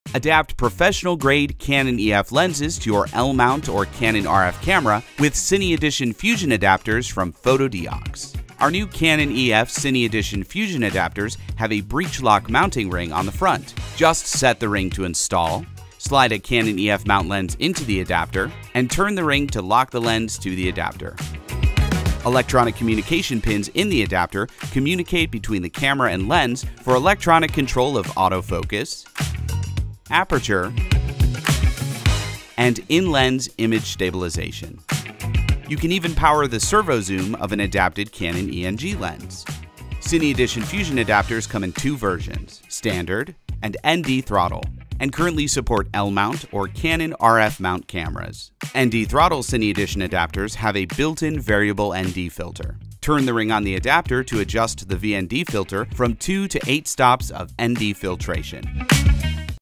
NEW Canon EF to L-Mount and Canon RF Cine Edition FUSION Adapters
Adapt professional grade Canon EF lenses to your L-mount or Canon RF camera with Cine Edition FUSION adapters from Fotodiox!